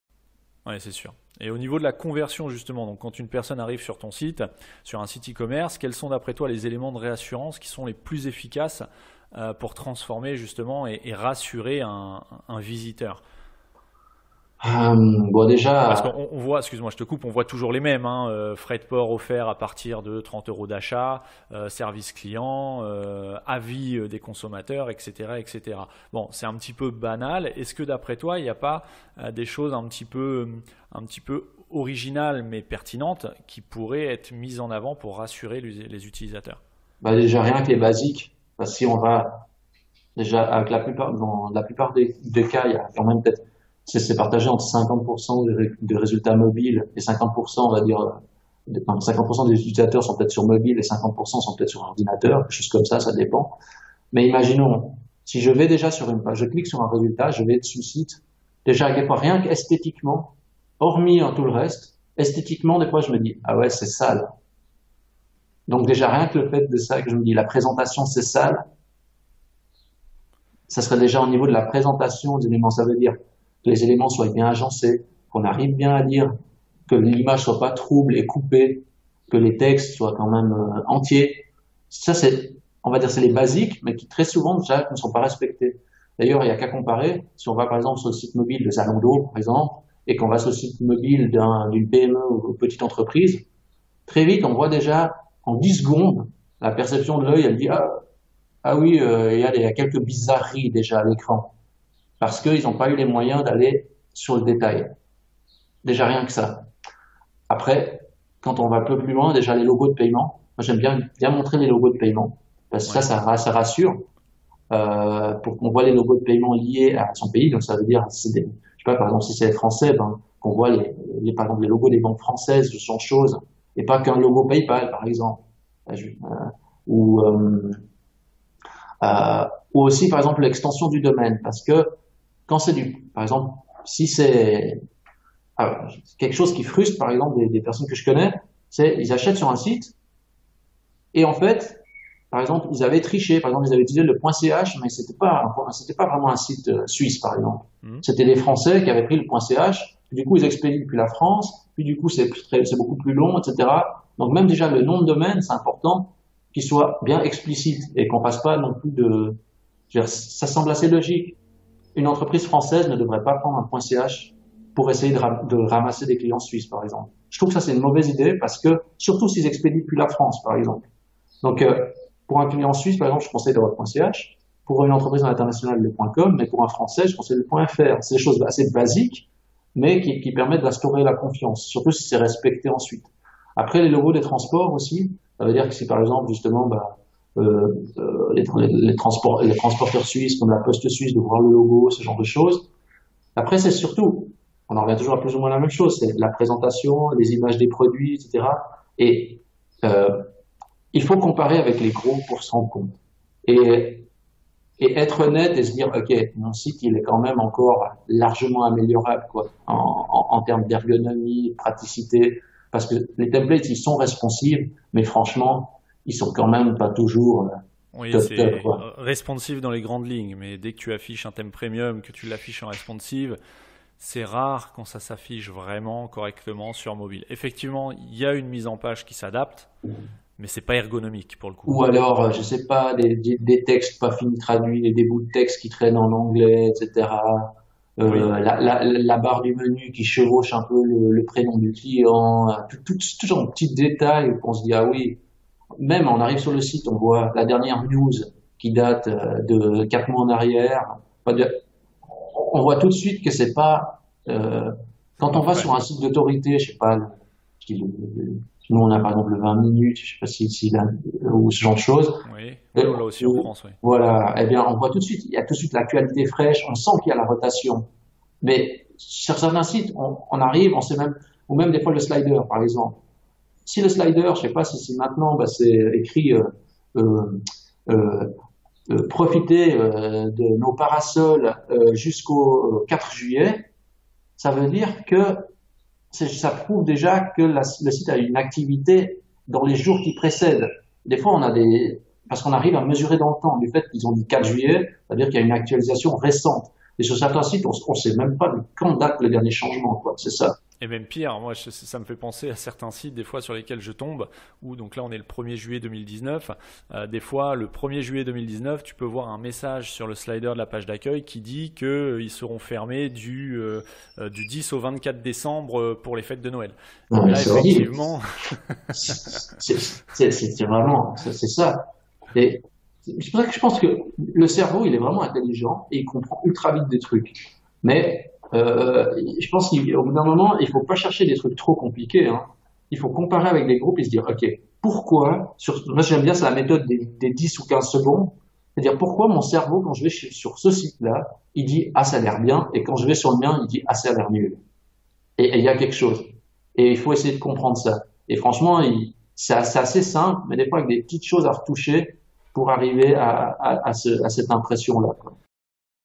Les élements de réassurance selon Germain Tenthorey de Webbax
## GUIDE GRATUIT ##